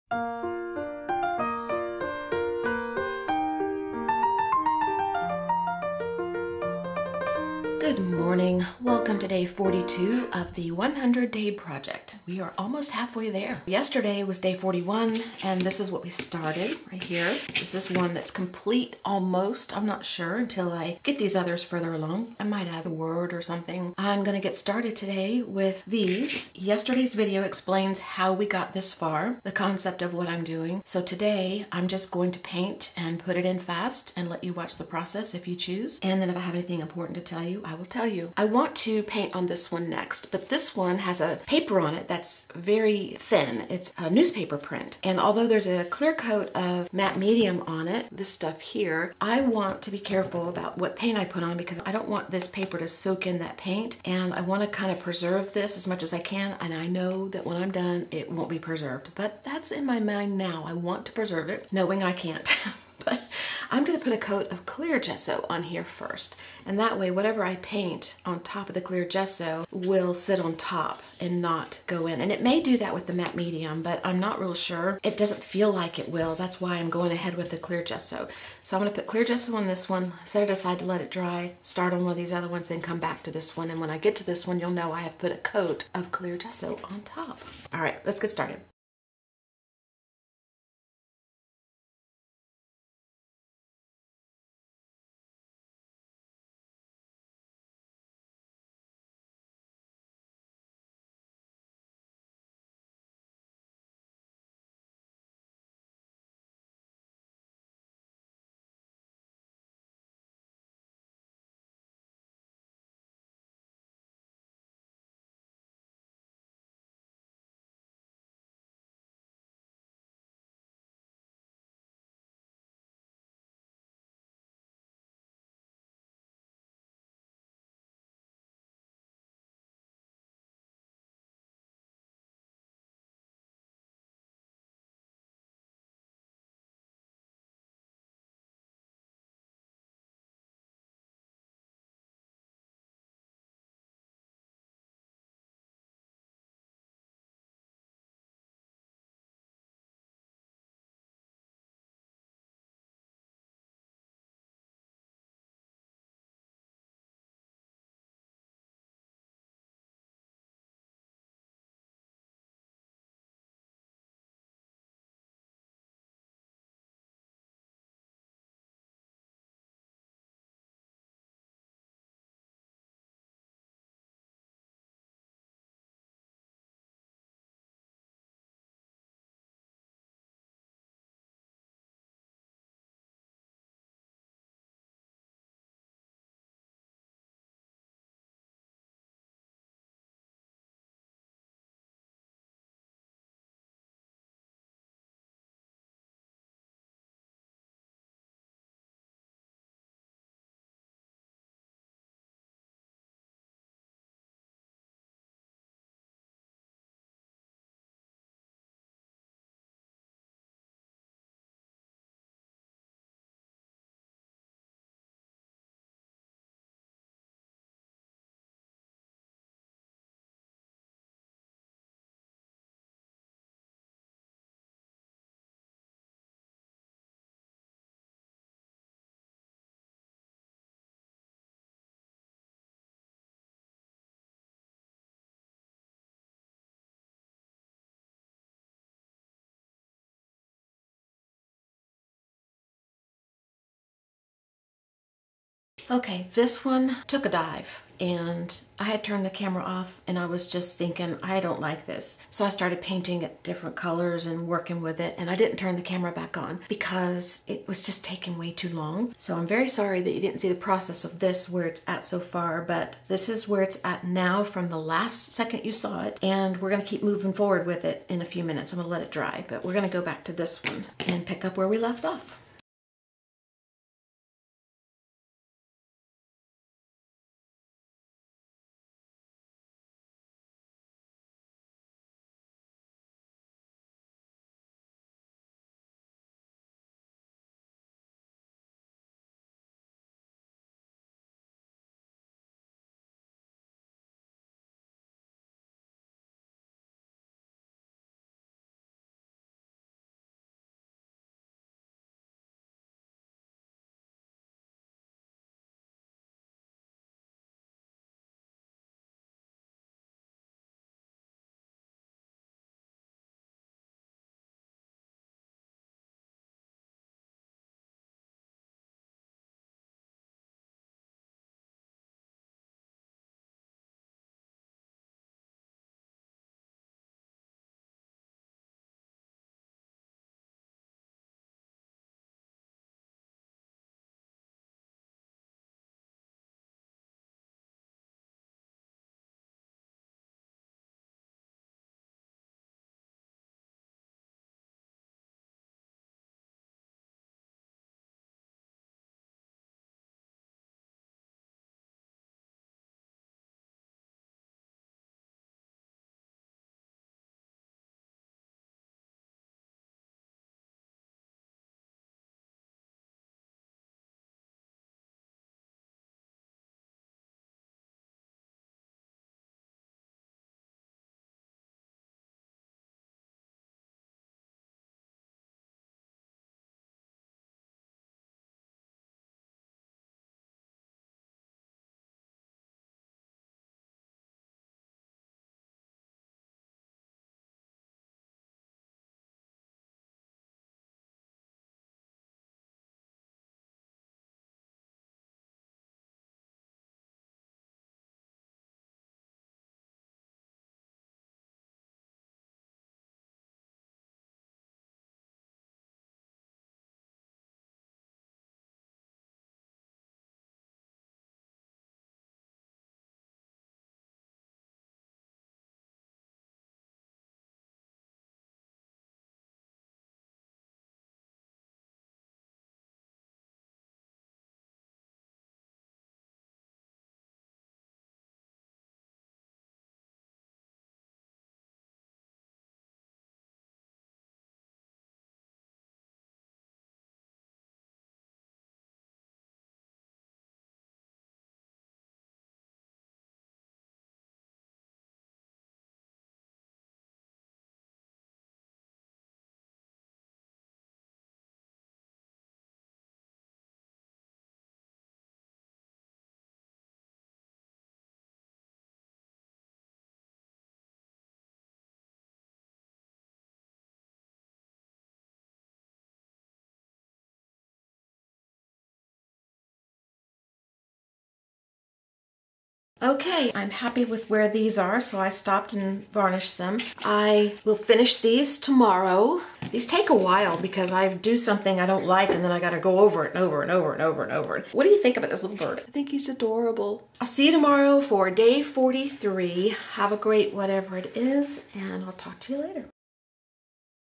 Part 2 - 5 Mini Canvases From Scraps - Day 42 The 100 Day Project
And then there were three. Three of the five mini canvases are 'done'. I might add a little something on them once all five are complete.  See you tomorrow for the final two canvases.